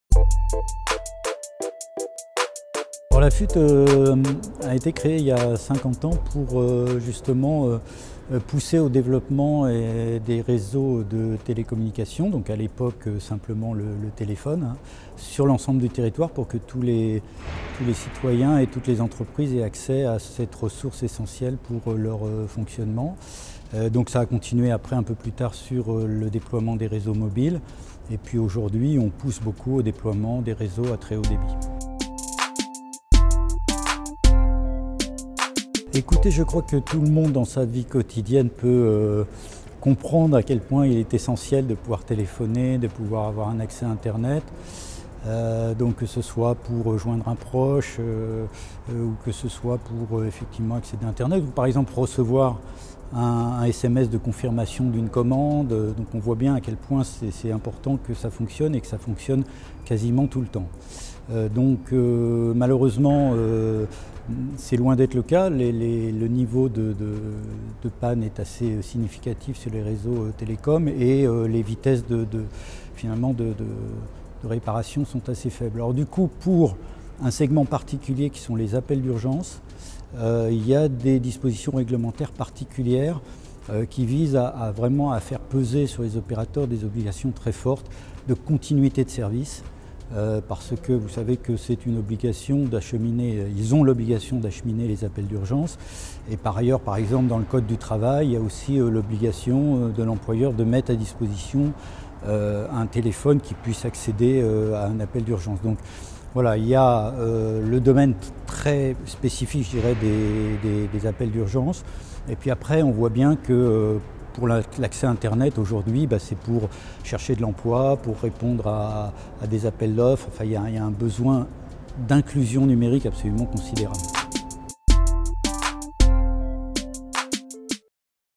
Bernard DUPRÉ (AFUTT) - infrastructures de réseaux - cese
Questions à M. Bernard DUPRÉ, vice-président de l'Association française des utilisateurs de télécommunications (AFUTT), auditionné par la Section des activités économiques du CESE dans le cadre de la saisine "L'impact des infrastructures de réseaux dans l'économie".